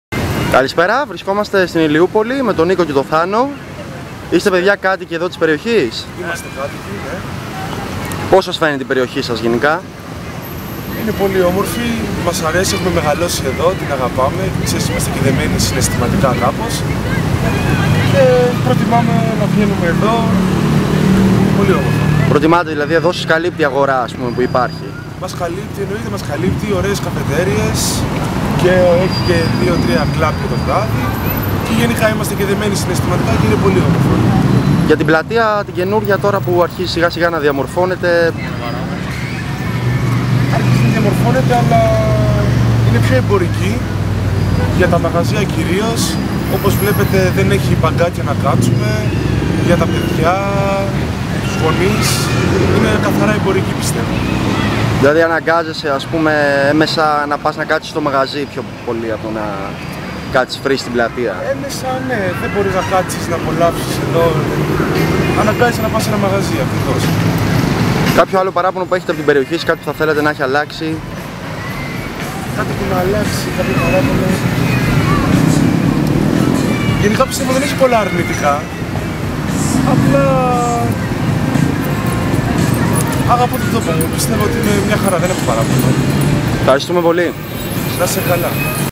Ηλιούπολη-Η πλατεία είναι καθαρά εμπορική
"Έχουμε μεγαλώσει εδώ είμαστε δεμένοι με την περιοχή μας,υπάρχουν αρκετά μαγαζιά για διασκέδαση.Η πλατεία όμως έχει γίνει καθαρά εμπορική δεν υπάρχει χώρος για να κάτσει ο κόσμος ελεύθερα αναγκάζεται να πάει σε κάποιο μαγαζί".
Νίκος και ο Θάνος κάτοικοι Ηλιούπολης.
ΒΙΝΤΕΟ:Μάνθος Ζαμπίτογλου